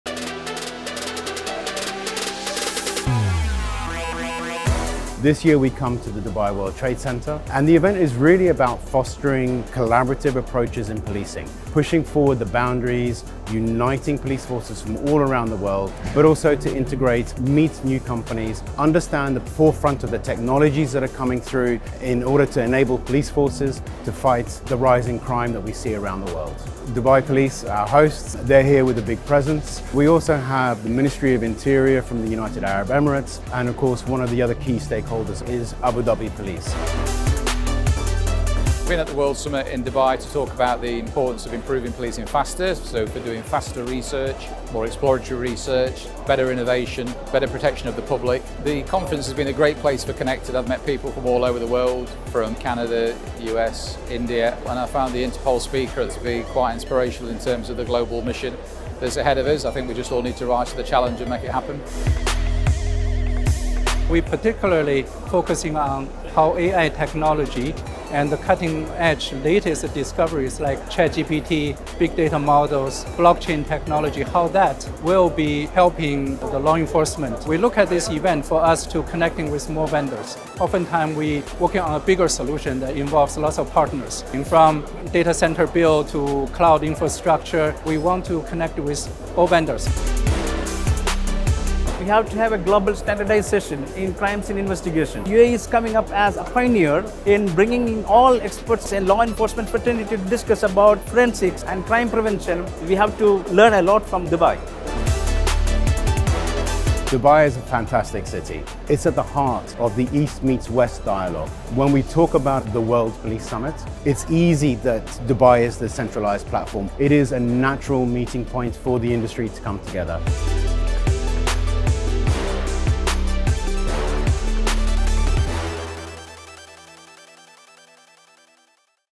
World Police Summit 2023
The 2nd edition of the World Police Summit took place this week at DubaiWorldTradeCentre, offering a platform for international as well as regional professionals in law enforcement and security to exchange industry innovations and strategies aimed at protecting the public.

In this video, we hear from:

Jean-Phillipe Cossé, Senior Vice President, Middle East – DMG Events
Dr John Coxhead, SFHEA, Professor of Policing Innovation & Learning – Royal Docks School of Business & Law, London
Xiaodong Zhou, Chief Technical Officer – Presight.ai
Reject Paul, Assistant Professor, Forensic Science Programme - Kerala Police Academy